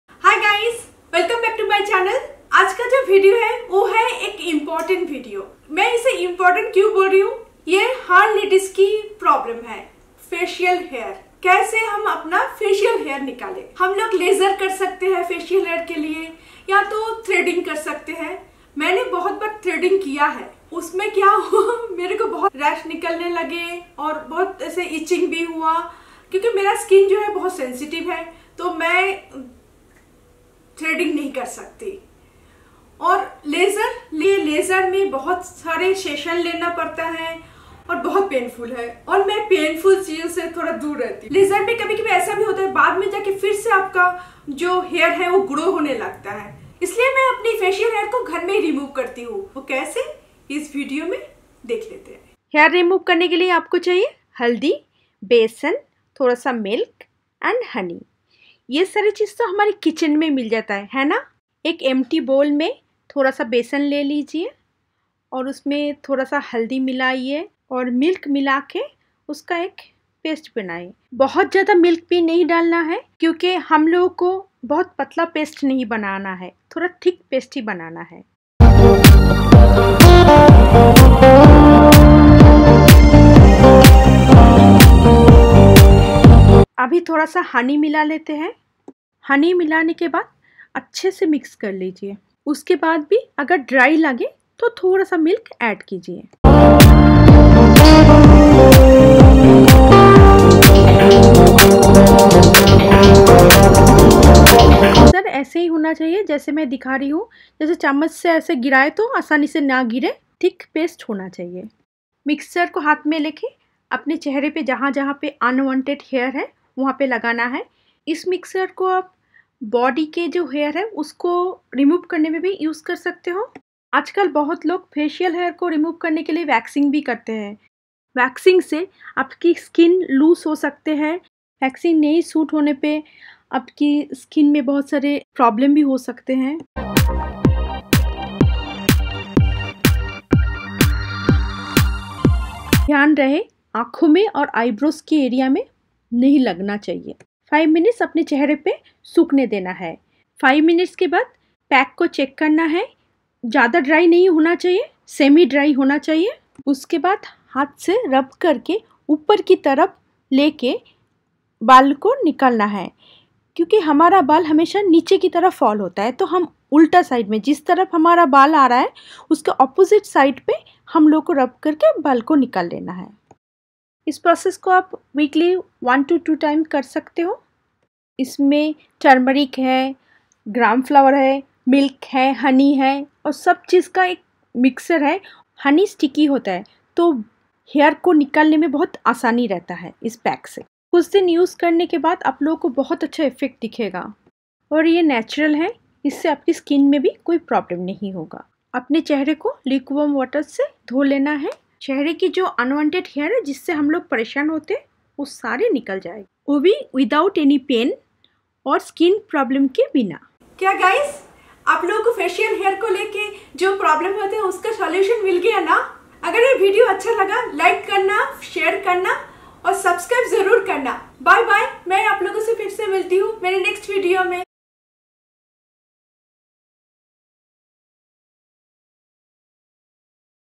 HOW TO REMOVE UNWANTED FACIAL HAIR AT HOME | QUICK & EASY HOME REMEDIES
This video is about how to remove UNWANTED FACIAL HAIR using simple home remedies